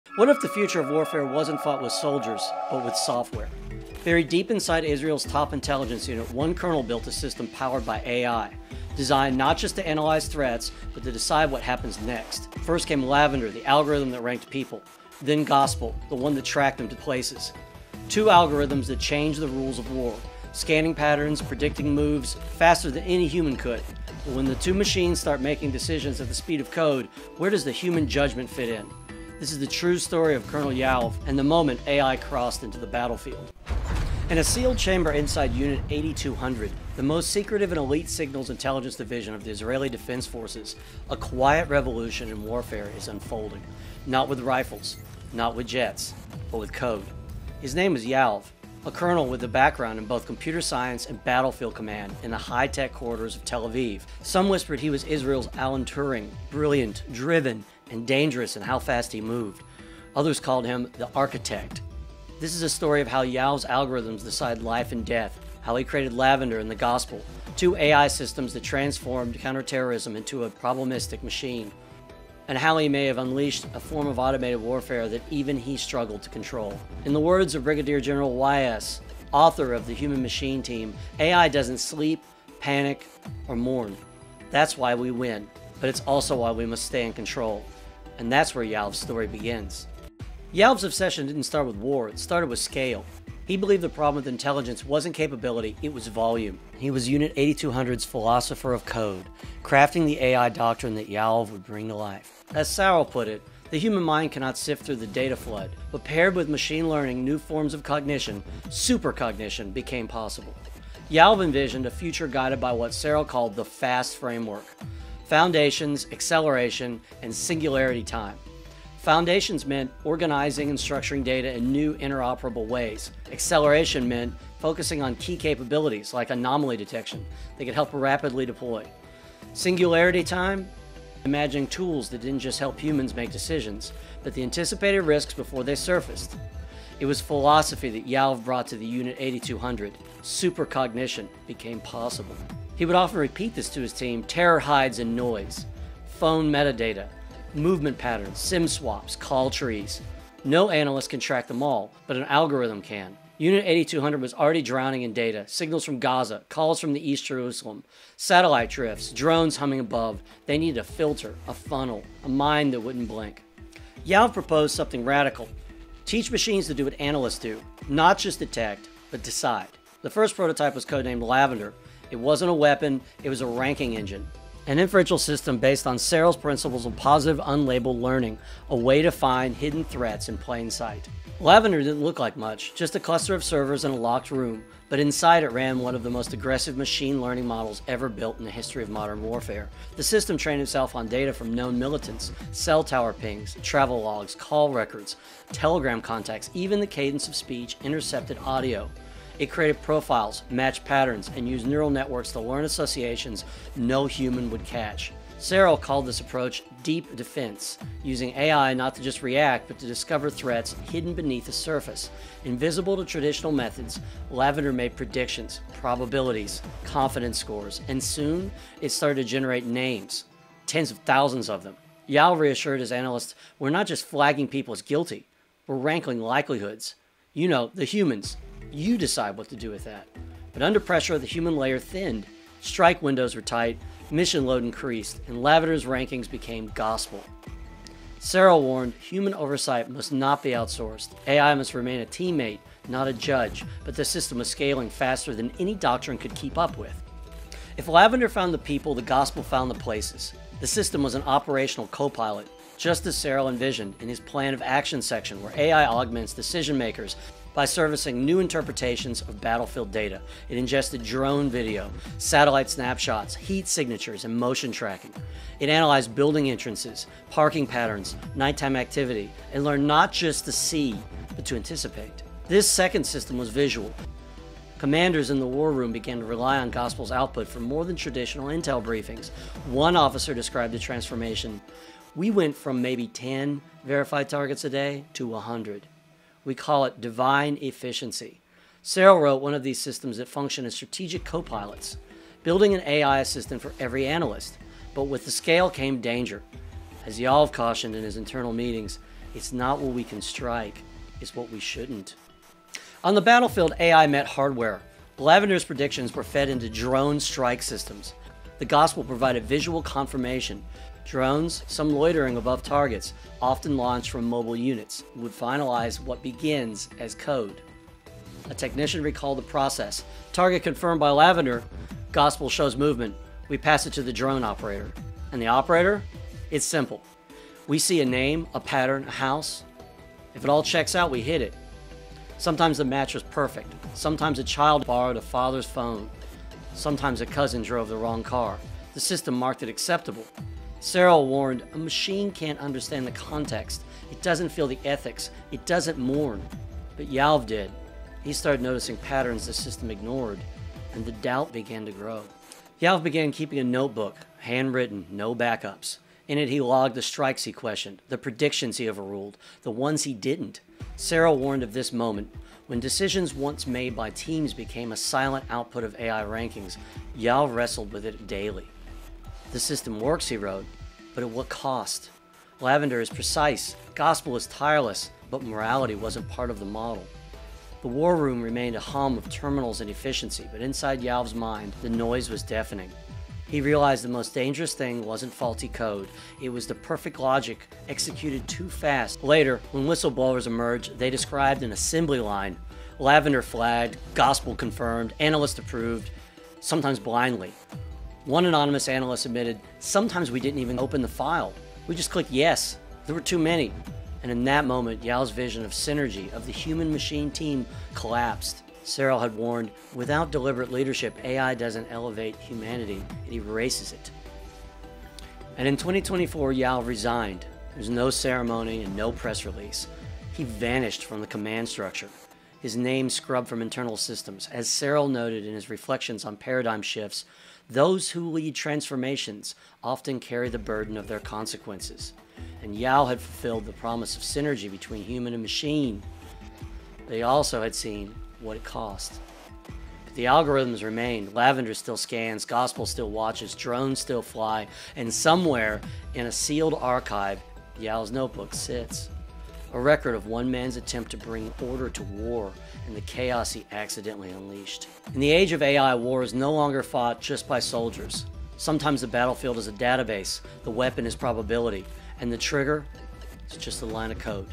The Secret Cyber Warfare AI Israel's Top Unit Uses Daily
Join me as I uncover the secret cyber warfare AI used daily by Israel's top unit, a game-changer in modern warfare. With Israel being a global leader in military technology, their AI-powered drones have become a decisive advantage on the battlefield. But how does Israel use AI-powered drones to win the war? From tactical strikes to drone warfare, I'll delve into the latest military news and explore how AI is revolutionizing combat. From the future of combat to tactical robotics, I'll examine the latest developments in Israel's military arsenal. With the threat of AI war with Iran looming, understanding how Israel is using AI to stay ahead is crucial. Get an inside look at Israel army's latest AI updates and how AI is helping Israel stay ahead in the war on terror.

00:00 - Intro
00:31 - Inside Unit 8200: The AI War Room
00:53 - The Architect: Who Was Yoav?
01:11 - Building the Machine: How the AI Works
01:36 - Too Much Data, Not Enough Humans
02:11 - Supercognition and “Singularity Time”
02:49 - Lavender Becomes Gospel
03:34 - The AI That Targets Humans
03:52 - The Collapse of Human Oversight
04:06 - Gospel: AI for Visual Confirmation
04:46 - Striking Without Thinking
05:14 - From Code to Target Confirmation
05:27 - Collateral Logic
05:52 - Yoav’s Notebook
06:15 - Morality Wasn’t in the Model
06:29 - The Assembly Line of Human Targeting
06:54 - The Resignation
07:16 - The Legacy That Remains